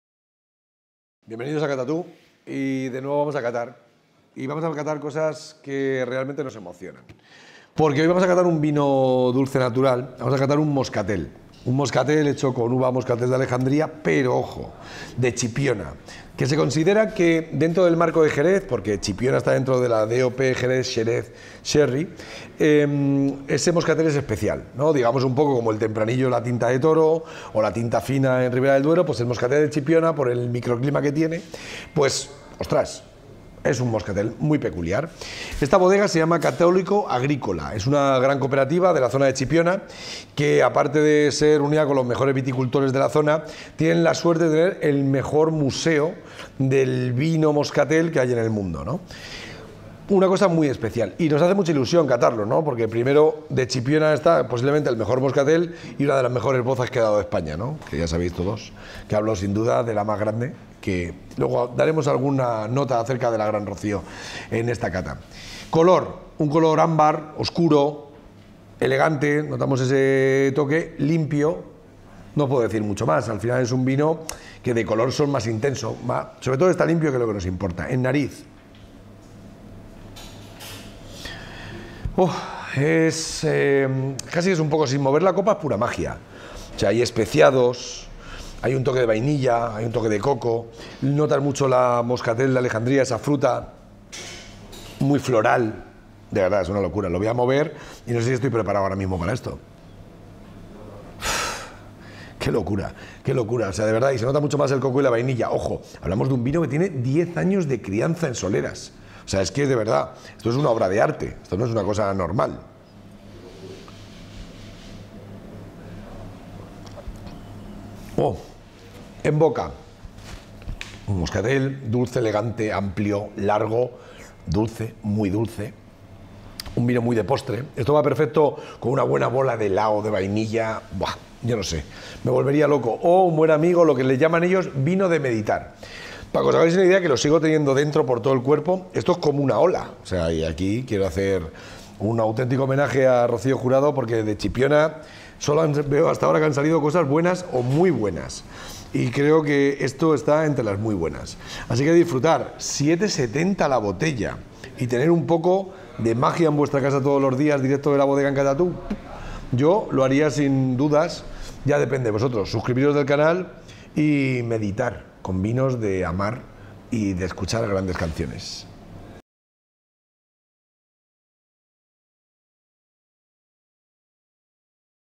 Vídeo Cata del vino dulce Los Madroñales Moscatel Selecto, Moscatel de Chipiona.
Las más selectas sensaciones del Moscatel de Chipiona

Solo las mejores uvas pueden ser un buen moscatel, pero solo los mejores moscateles pueden llegar a embotellarse como Los Madroñales Selecto. Eso y 10 años en las soleras de la bodega para que por evolución natural adquiera su característico y único color caoba muy oscuro y la aparición de complejos aromas que lo hacen irrepetible.

Aterciopelado y profundo. Diferente y, por supuesto, dulce a través de los aromas de vainilla, coco, chocolate, café, higos, regaliz y un sinfín de suaves y delicados matices. Las más selectas sensaciones del Moscatel de Chipiona

Medalla de oro. Challenge International du Vin.